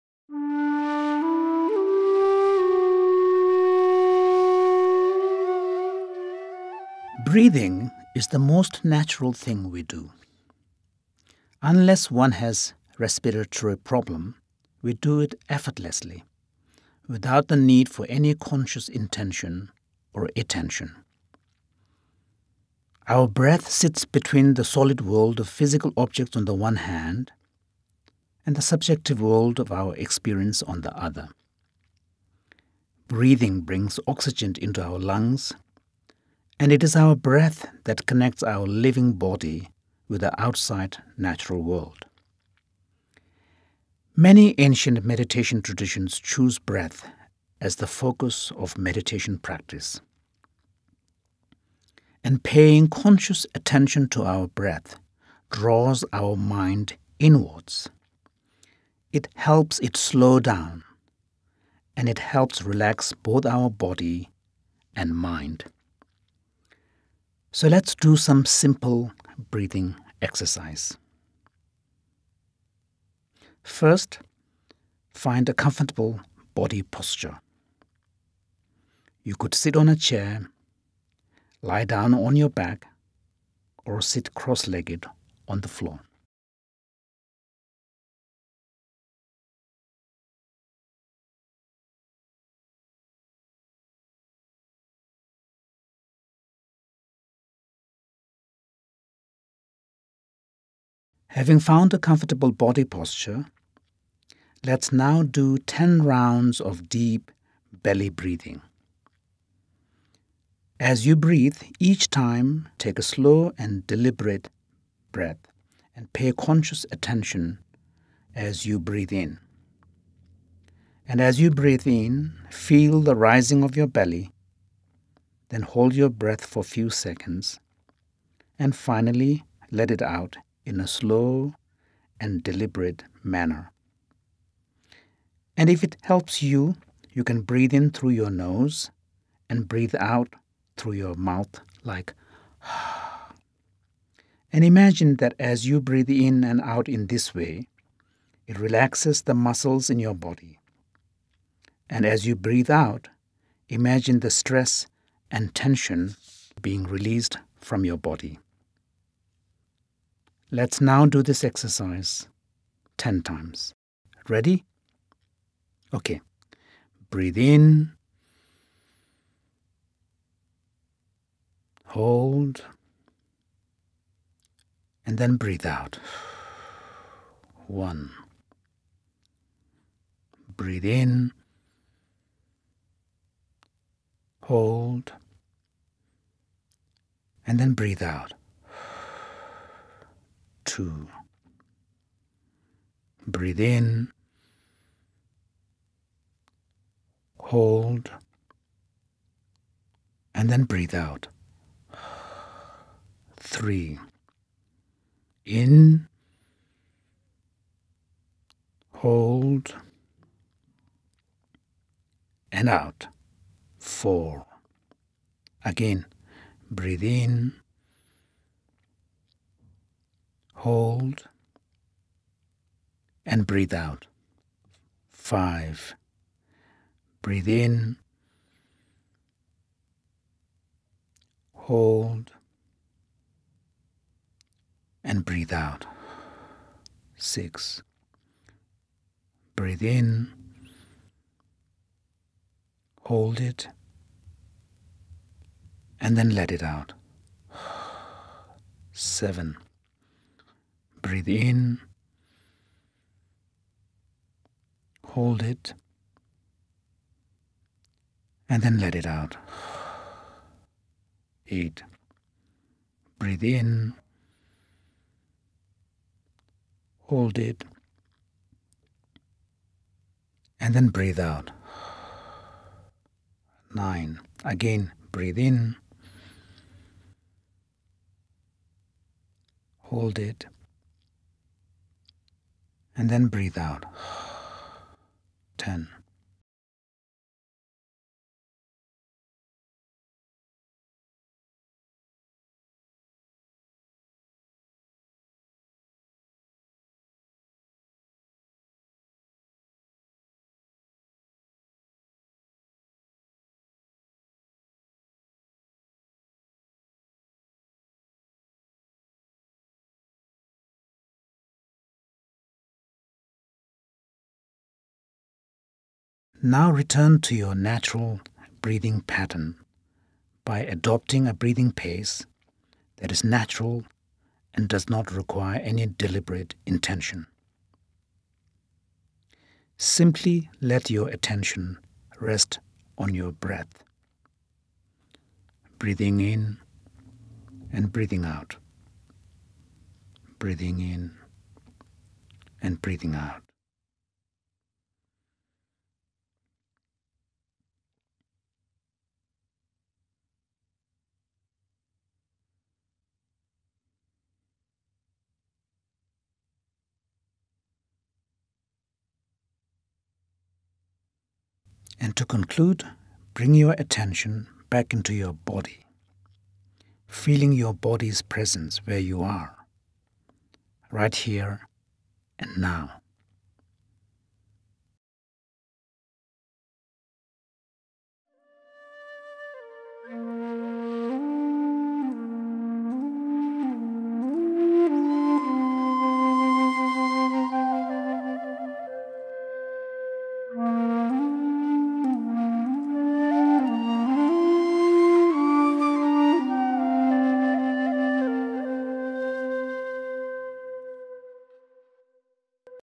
Basic Meditation: Simply Breathe - Thupten Jinpa on The Mandala App
In this meditation, learn with Dr. Thupten Jinpa how to breathe and be conscious about it.
This meditation is released free of cost for the benefit of all, to tide over the anxiety resulting from the coronavirus epidemic.